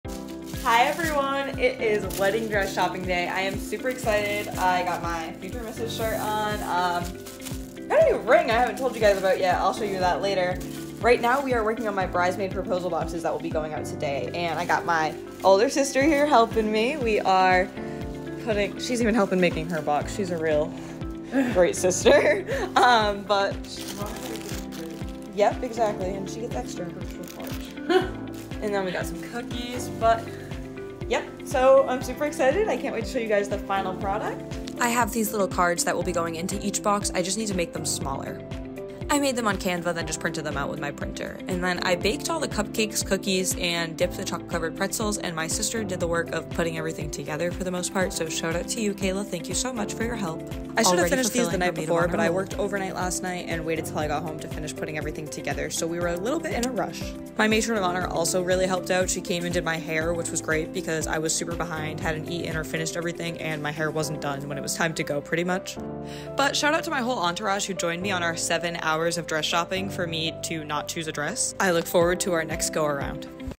Bridesmaid proposal boxes and wedding dress shopping!!
This is back from December 21st I foeuu go it to edit and post but this was such a fun (and long) day! I got home from my overnight shift and got to work on my proposal boxes for my bridesmaids, then we went dress shopping for 7+ hours! I’ll make a video explains how that went!! I’m super excited to Marry the love of my life next January!! Soon the year cont down will begin!!